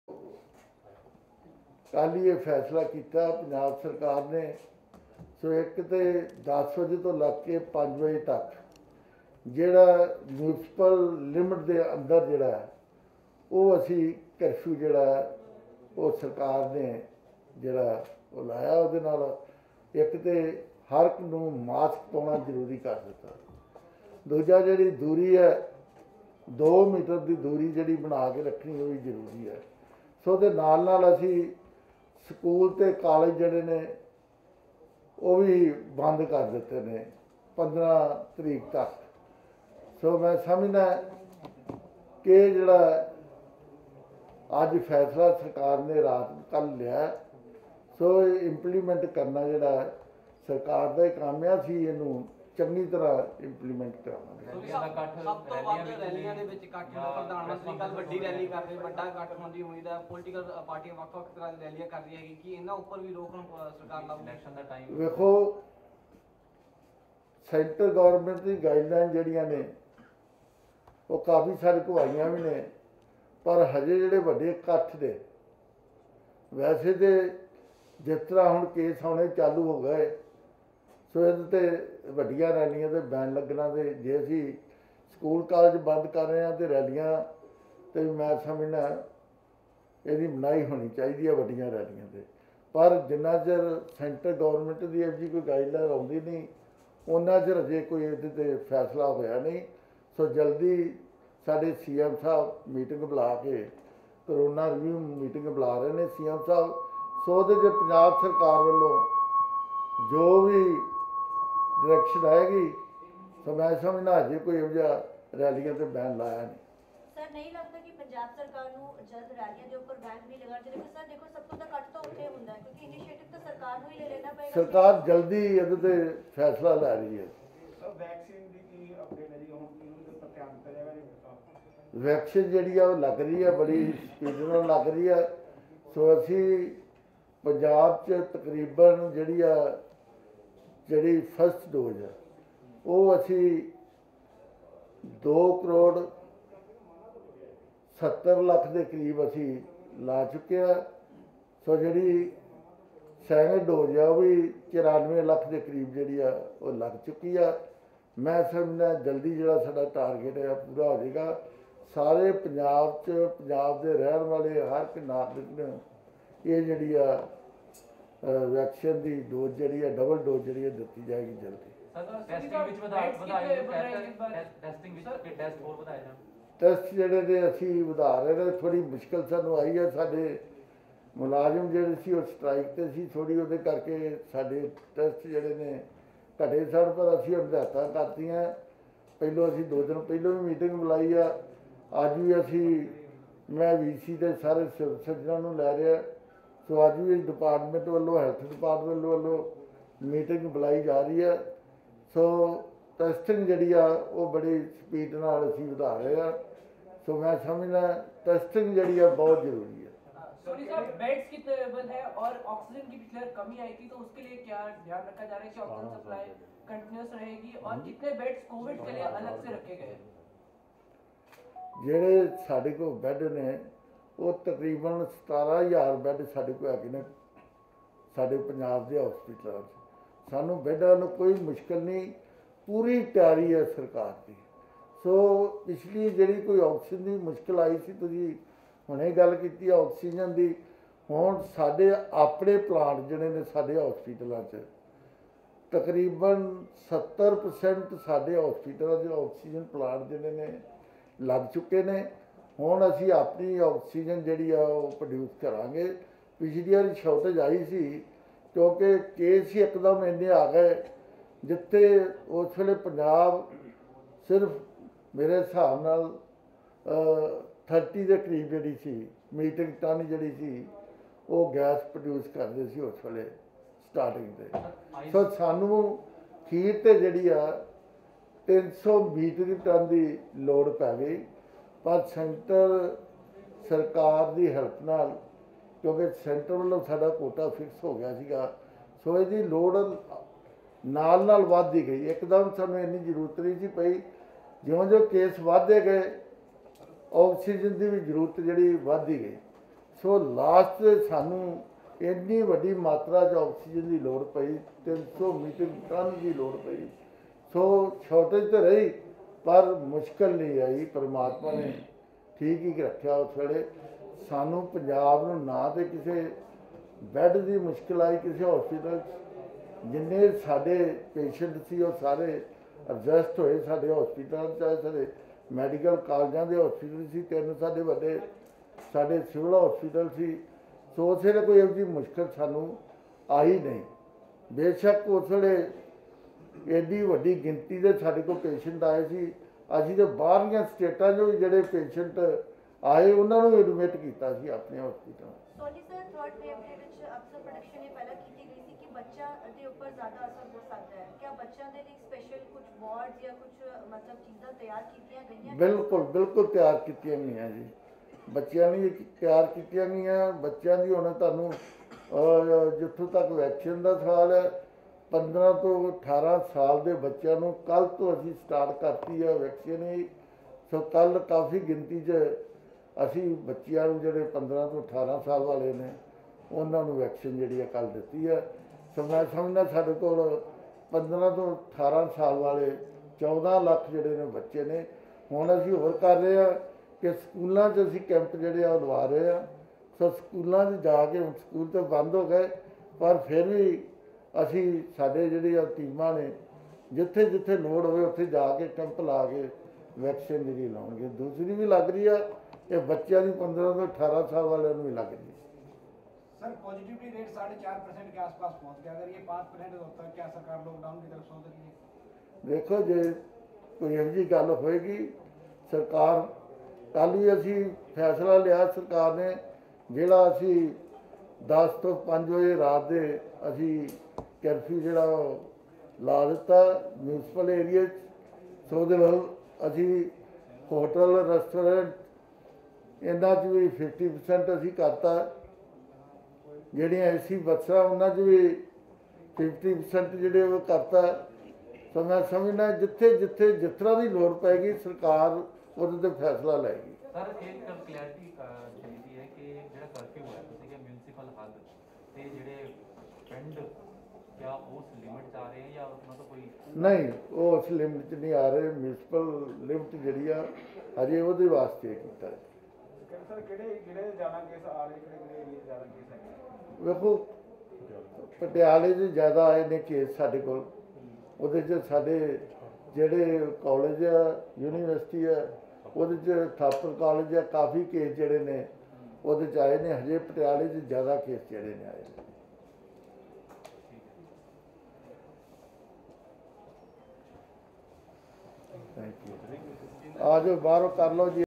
Center should ban election rallies-Deputy CM OP Soni
The Deputy CM OP Soni said: Center should ban election rallies; Why allow such a crowd when schools and colleges are closed?